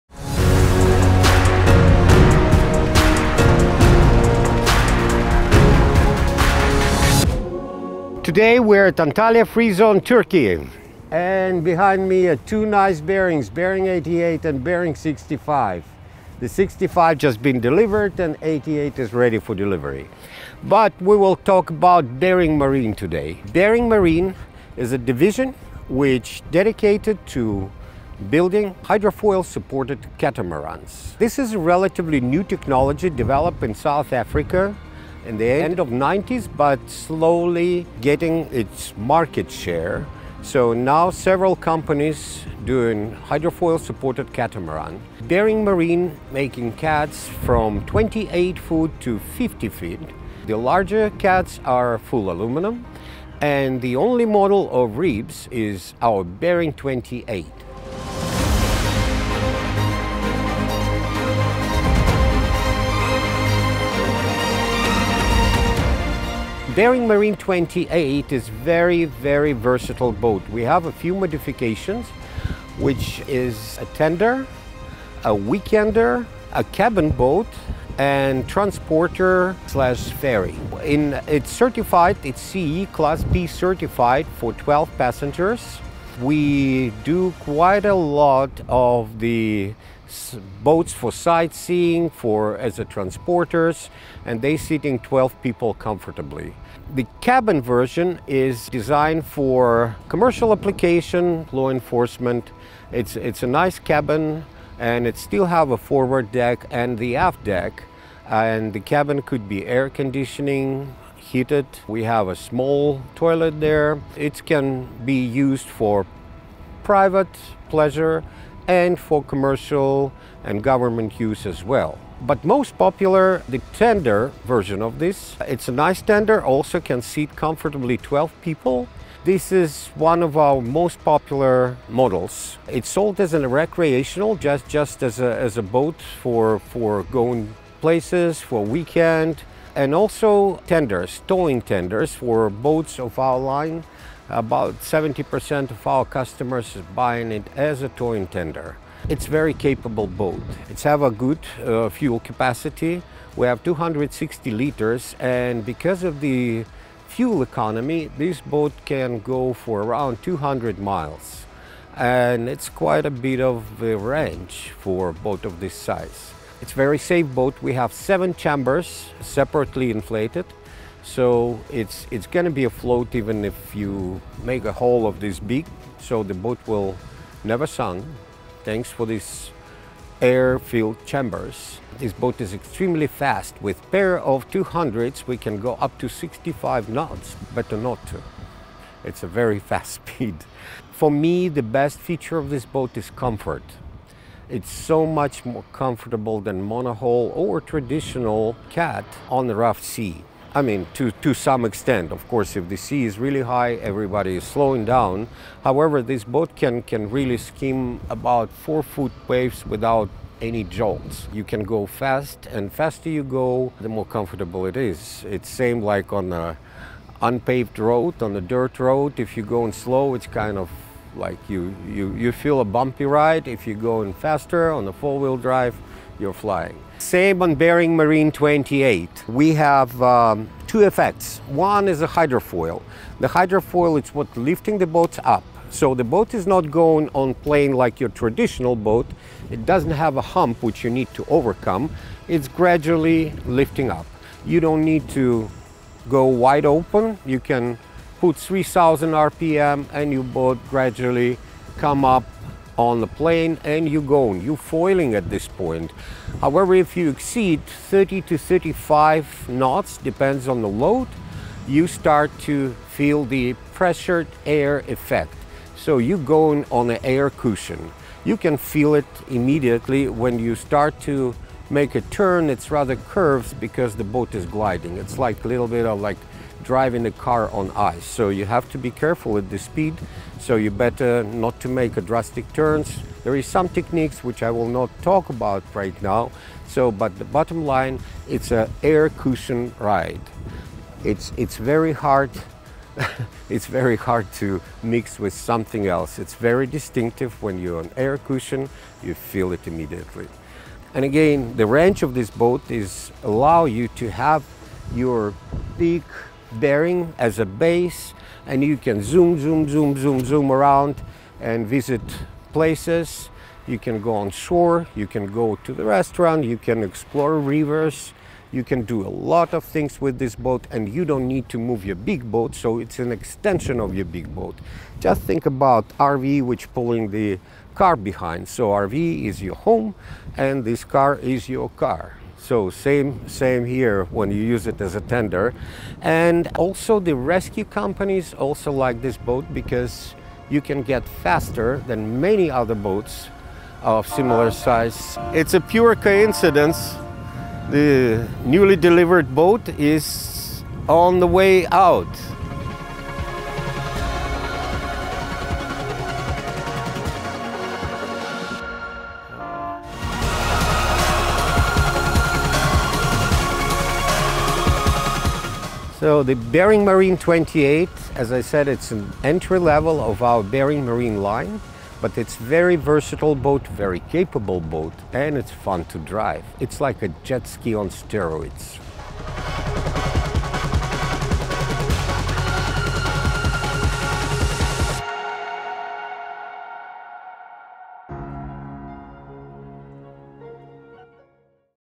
Bering Marine 28 – The Ultimate High-Performance Hydrofoil Tender!
Join Alexei Mikhailov as he takes you through the BM28, a cutting-edge tender in the Bering Marine lineup. With its lightweight catamaran hull, foil for reduced drag, and dual 200 hp engines, this powerhouse delivers top-tier speed, efficiency, and stability.

From its sleek design and vacuum-infused construction to its intelligent fuel system, ergonomic cockpit, and top-notch safety features, the BM28 is built for both adventure and reliability. Watch as it slices through the water, reaching speeds up to 52 knots with unmatched agility.